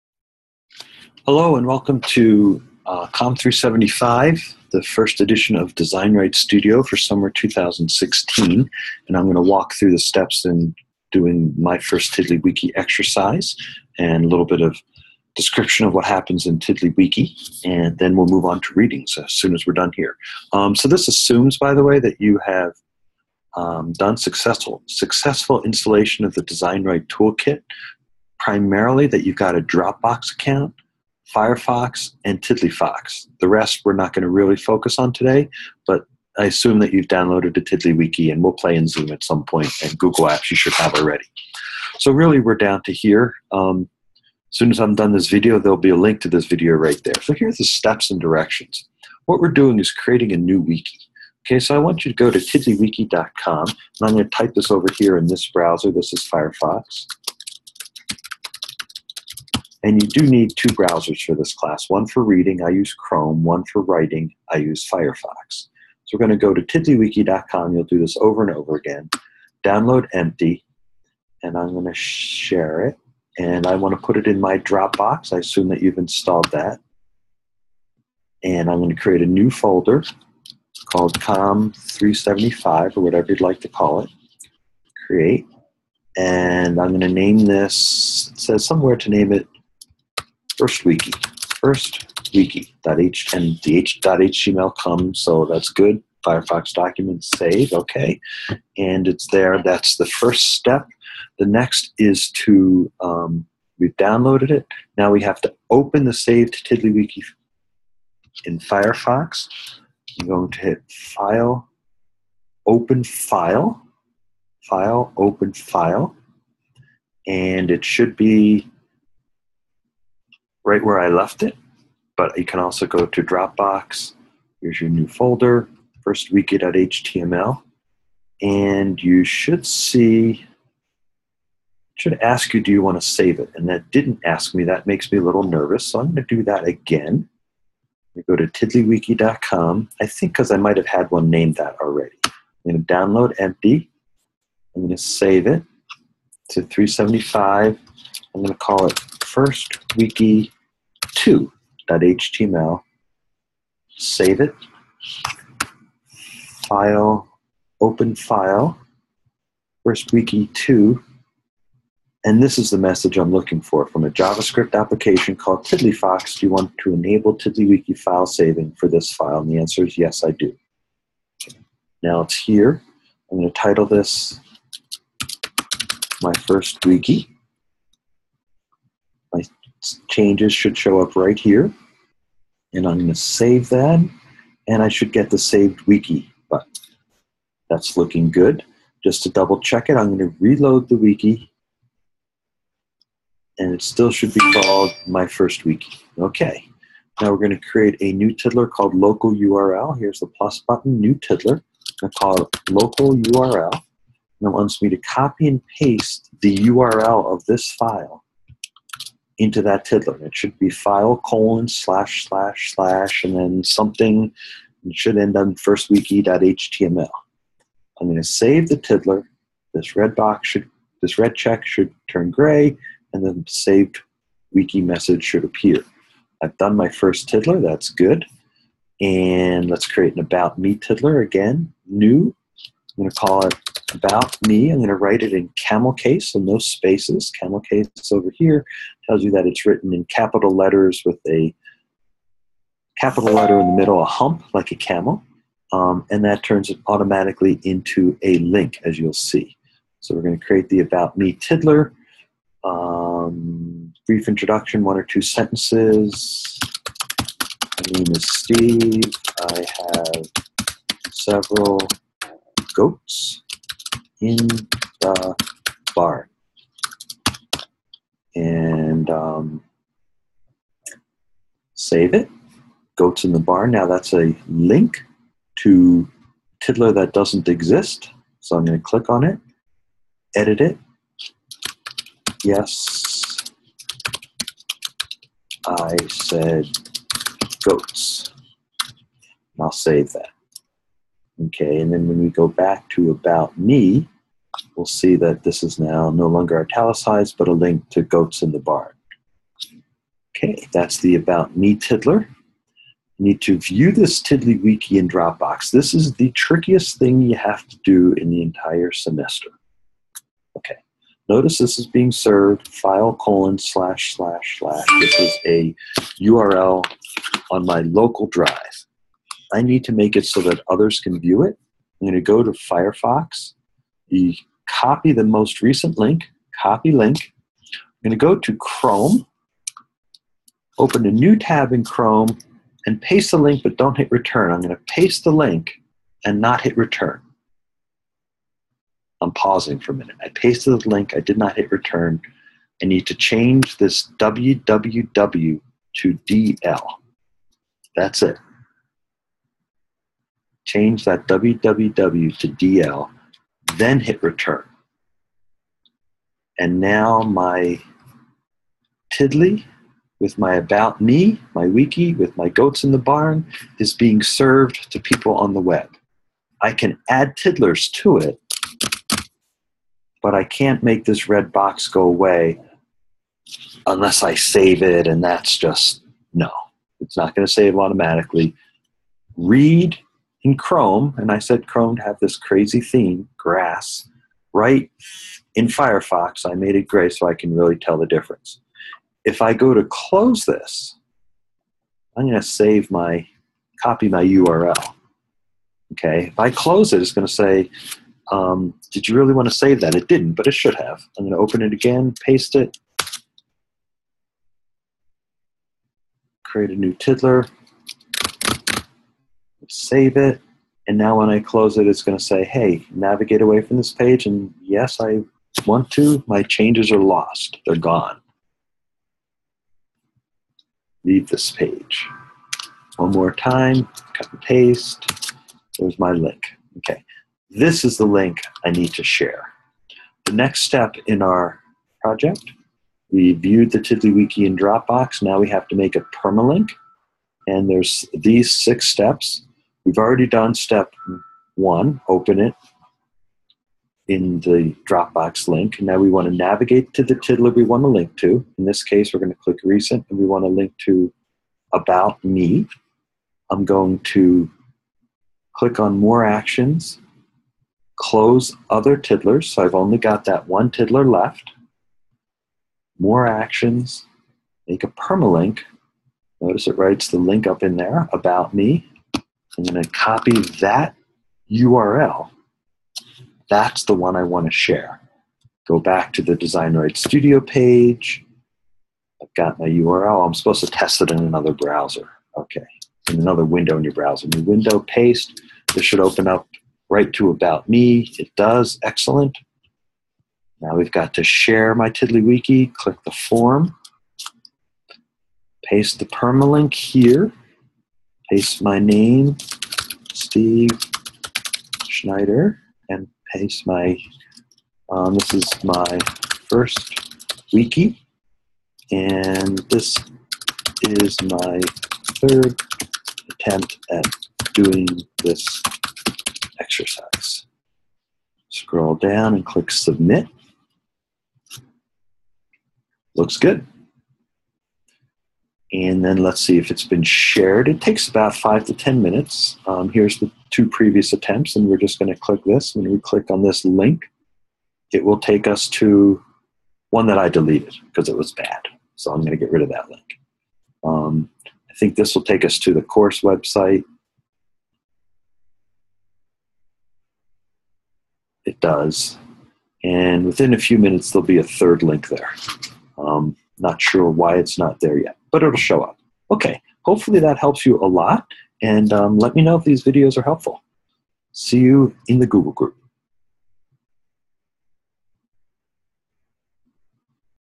HowToDo: MyFirstWiki Exercise
A step-by-step approach to doing the first wiki exercise in DesignWriteStudio summer 2016.